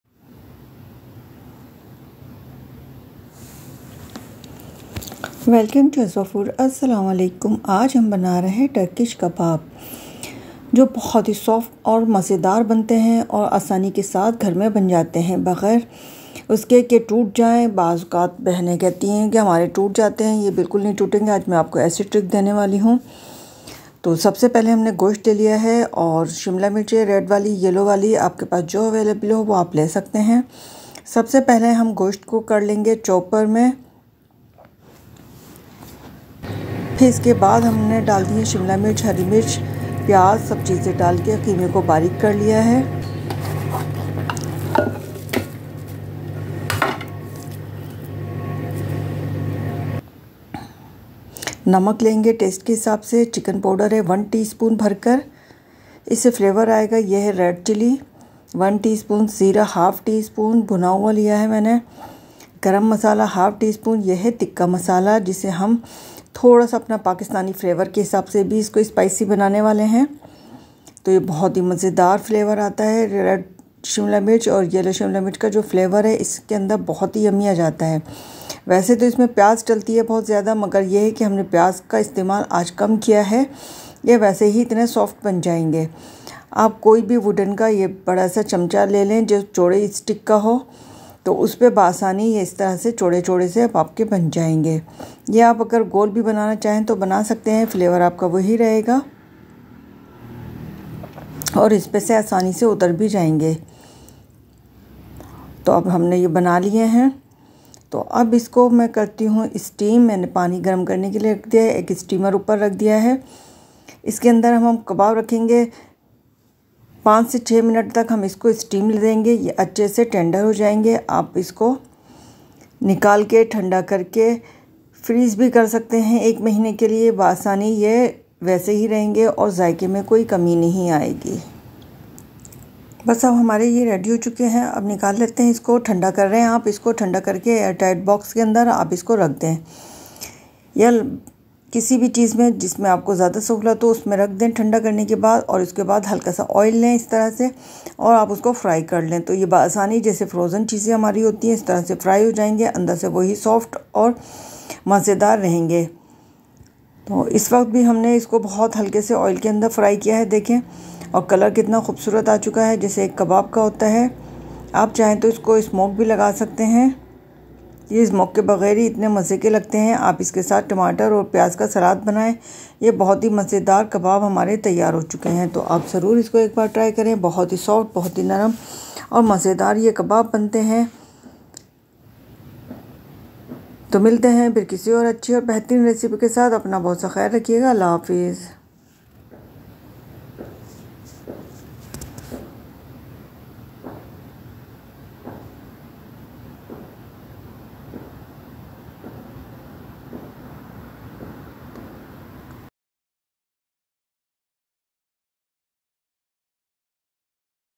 TURKISH ADANA KEBAB RECIPE | TURKISH KEBAB WITHOUT GRILL || byazwafood
TurkishAdana kebab / Urfa kebab with some special seasoning by azwafood

Azwafood channel

ingredients:
minced meat (Beef, lamb, chicken)  i used chicken thigh
onion medium size
red bell pepper medium size
coriander leaves tbs (chopped)
green chili & tbs or to taste (chopped)
chili flakes  Tbs
paprika powder tsp
salt 1 tsp or to taste
cumin powder Tsp
all spices powder 1
Tsp
chili powder 1/4 tsp or to taste
black pepper 1/4 tsp
clarified butter (ghee) 1tbs
garlic paste
 tb

Home Made Is The Best!

& hit 👍 LIKE button to appreciate my hard work. thank